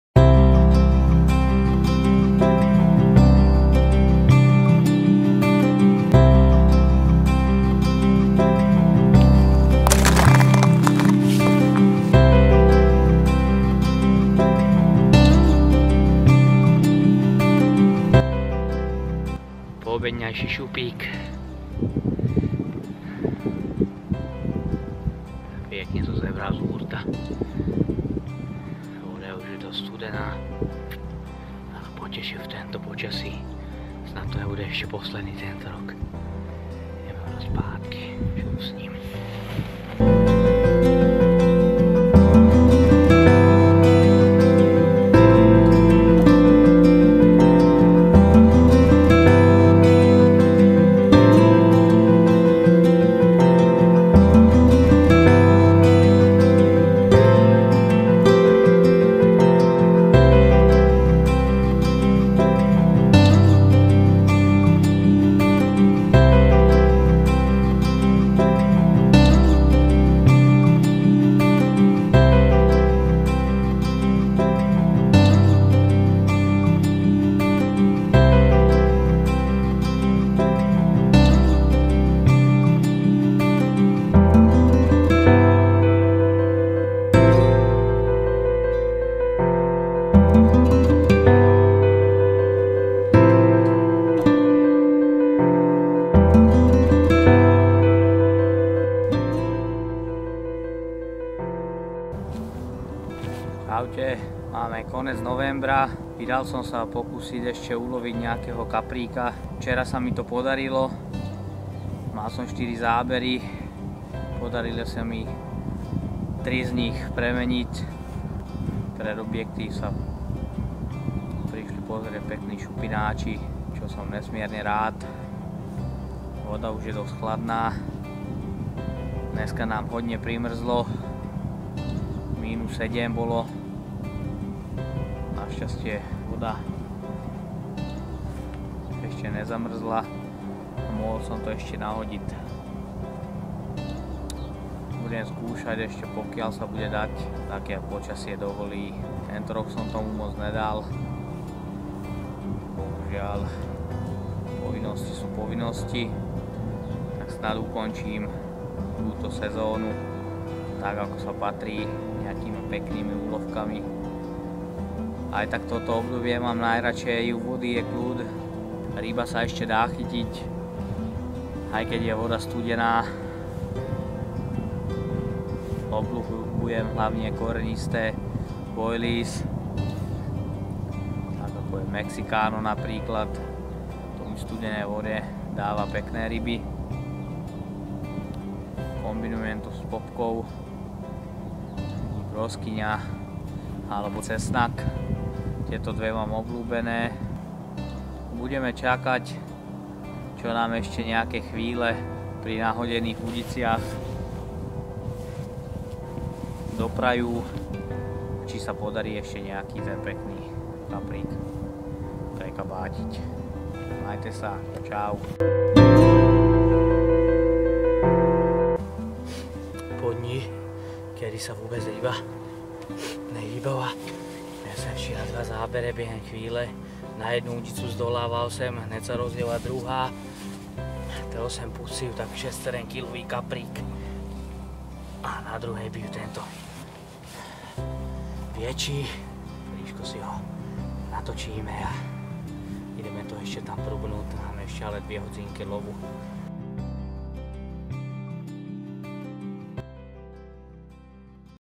NO RESPECT Kimot Team ● V Bode mrazu
Karáskové koncoročné vychádzky k vode, kde sa teplota pohybovala u bodu mrazu.

S pozdravom Váš NO RESPECT Kimot Team

Máš dotaz, alebo sa chceš len niečo súkromne opýtať?